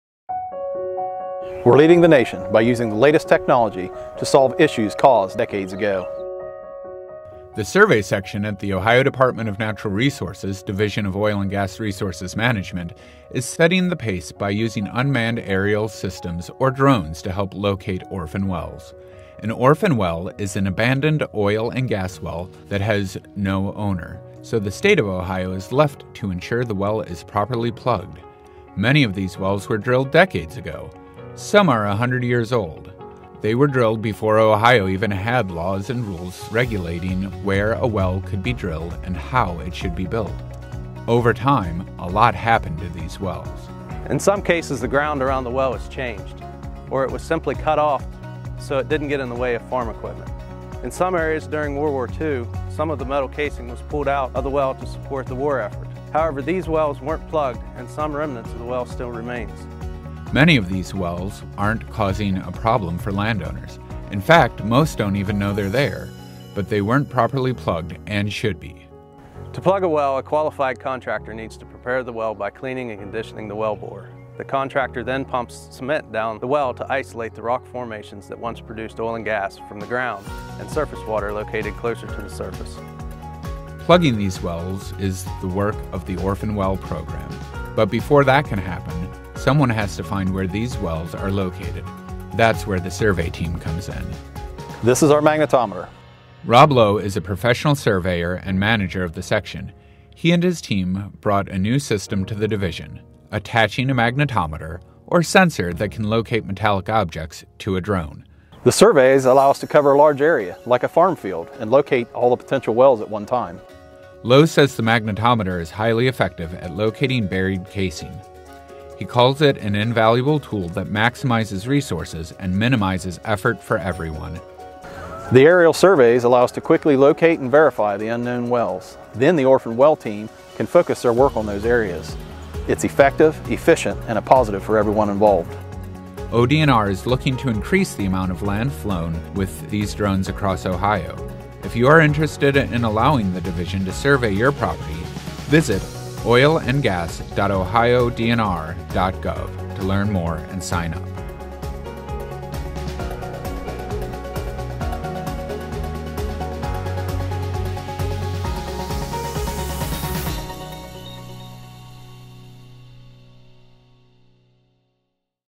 Ohio's Orphan Well Survey Program
Learn more about the impressive science the ODNR Division of Oil and Gas Resources Survey Section uses to try to locate Orphan Wells. Drones can find oil and gas wells from the air! Visit oilandgas.ohiodnr.gov to learn more about what Ohio is doing to address these wells.